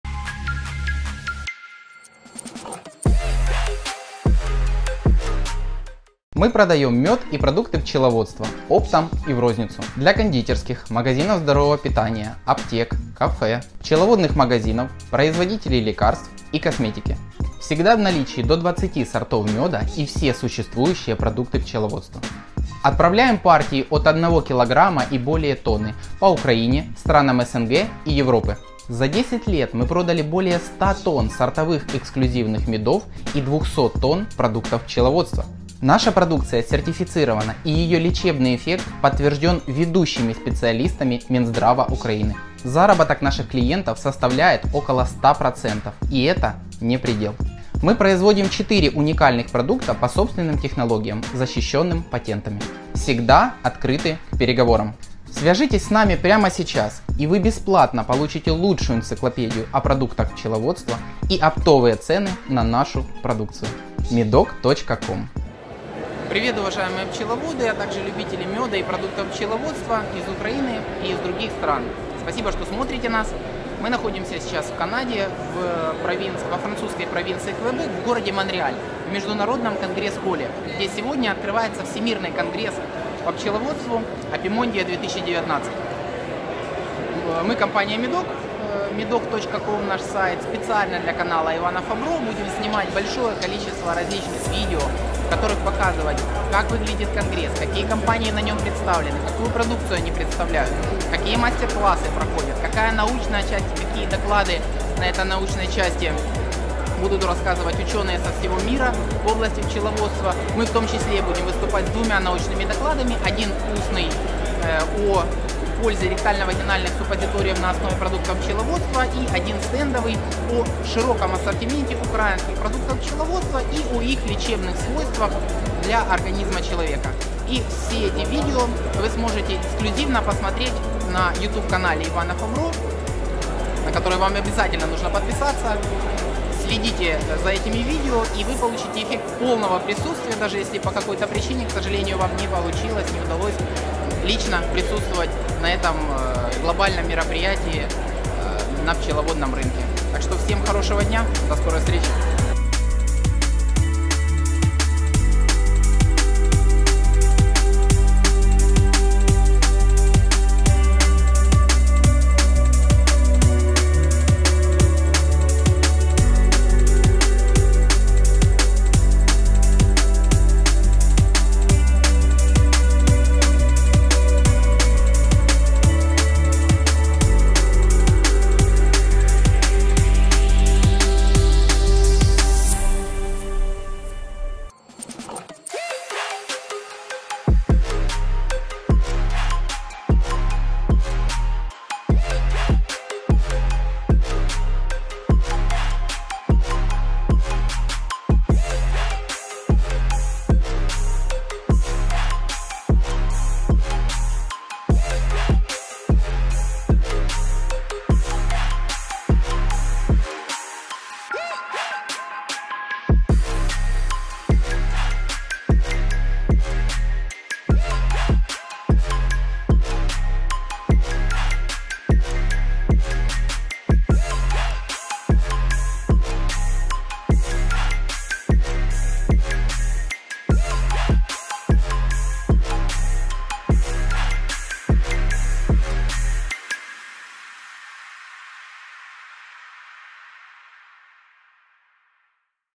Апимондия 2019 / Предисловие
Анонс видео с конгресса Апимондии 2019 в Канаде